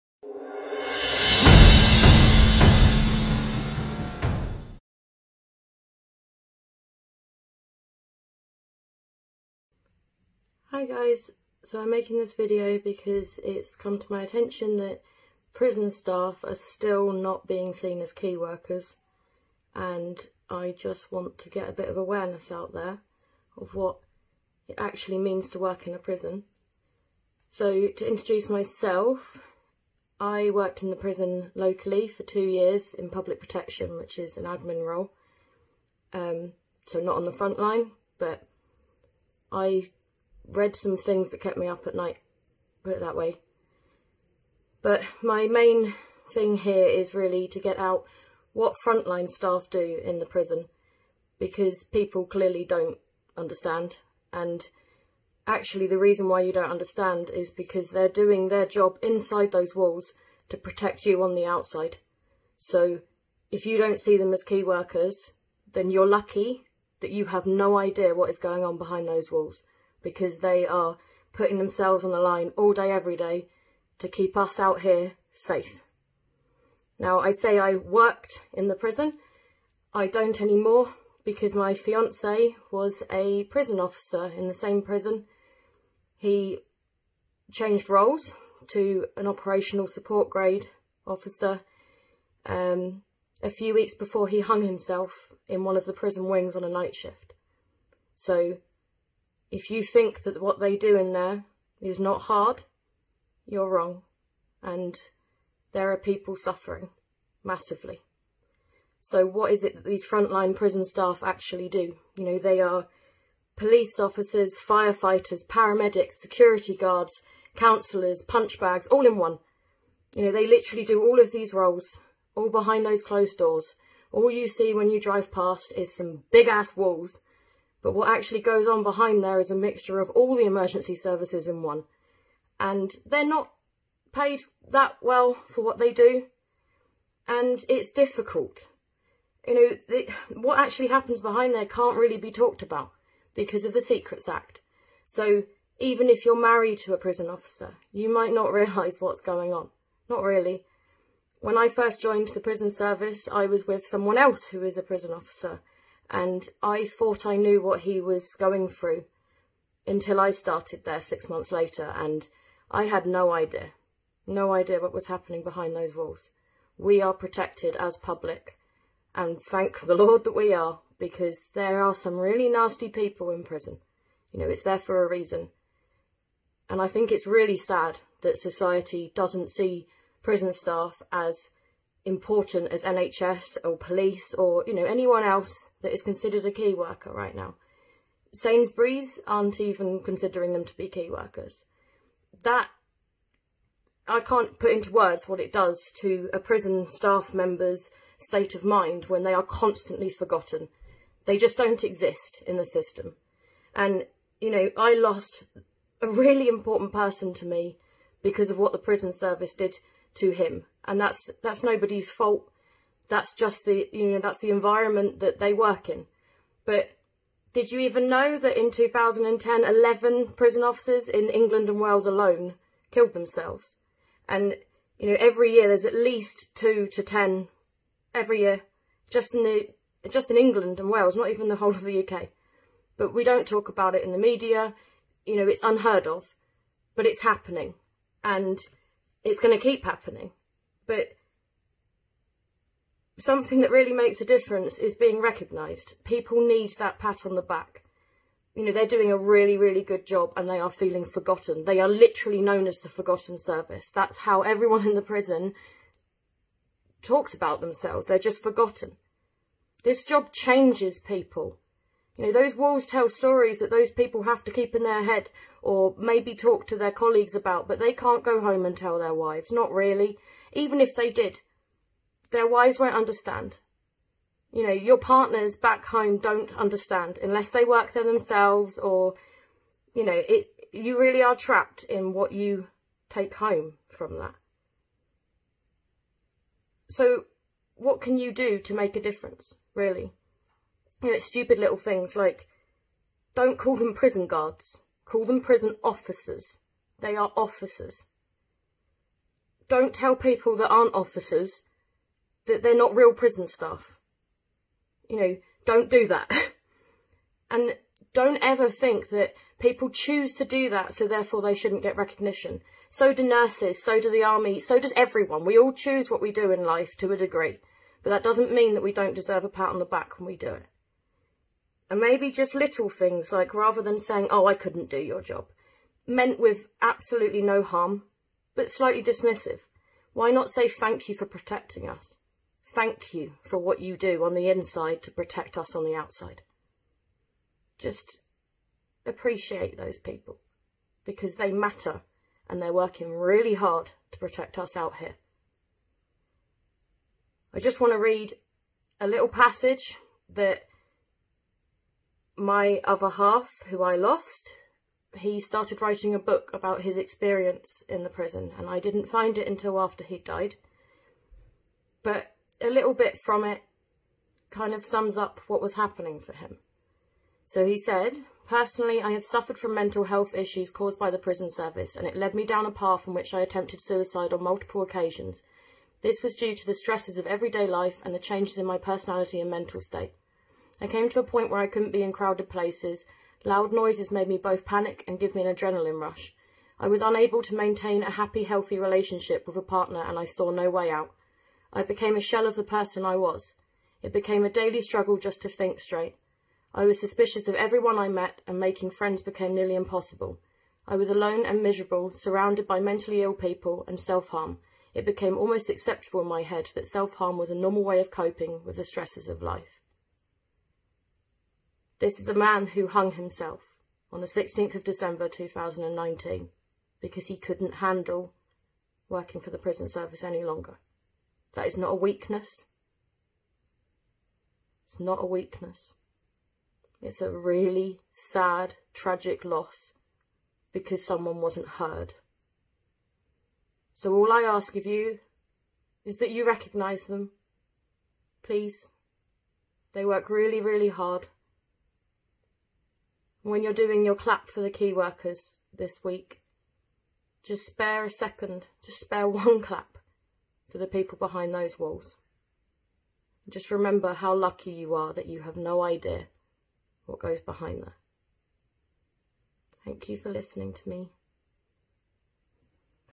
Prison officers: Powerful stuff from this former prison officer thoughts on what she says ?
My latest video this time its on Prison officers: Powerful stuff from this former prison officer thoughts on what she says ?